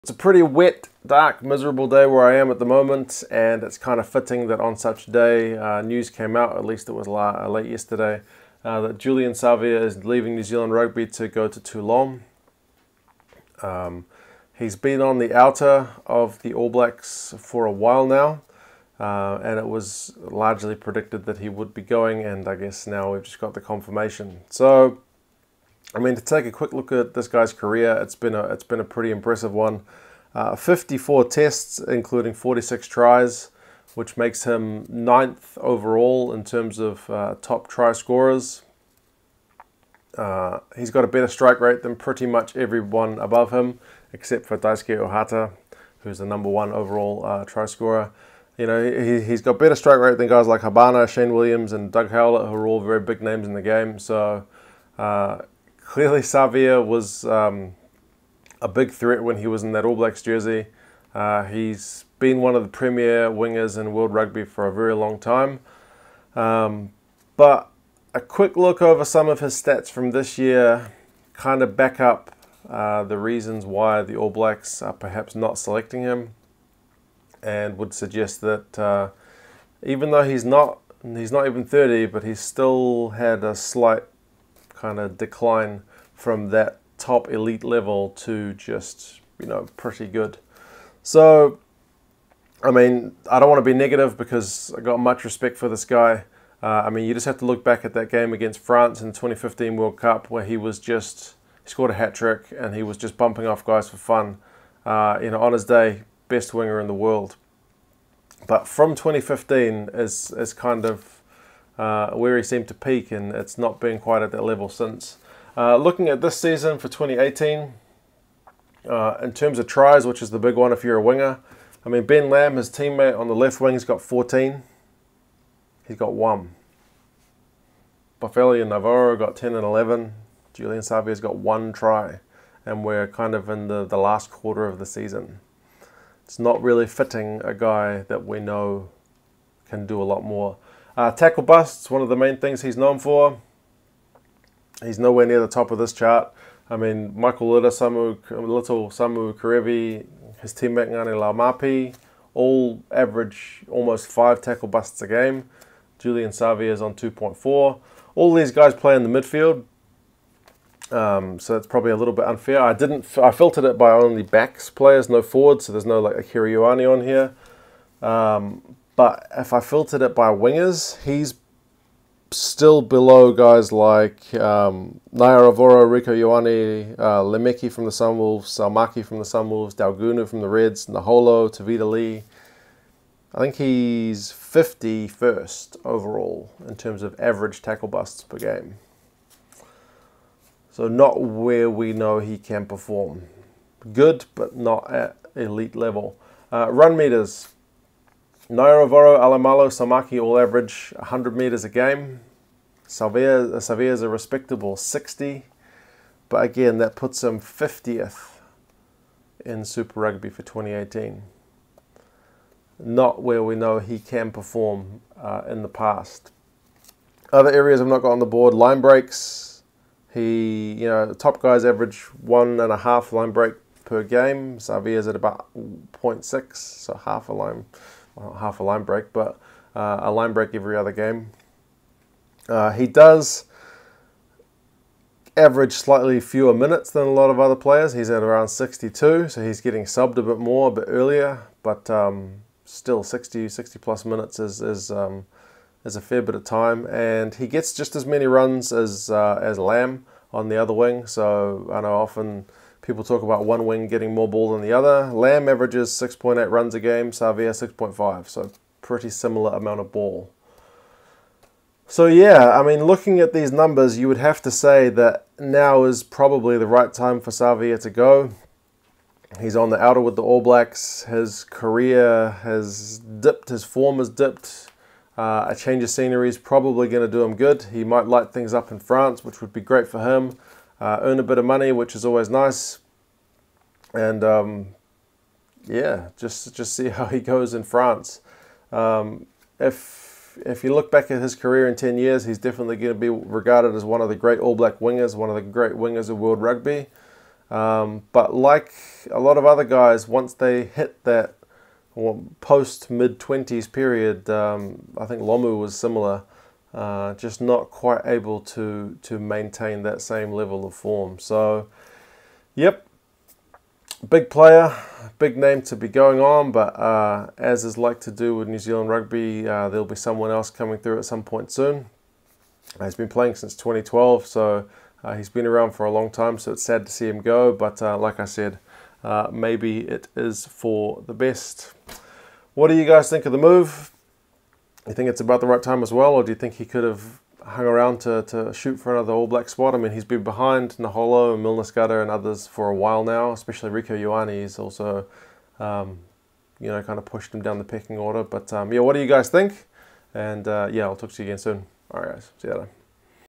Julian Savea leaving NZ to join Toulon
News out the Julian Savea is leaving NZ rugby.

*I should mention I said a word about Jonah Lomu is this vid but didn't mention his health issues affecting his career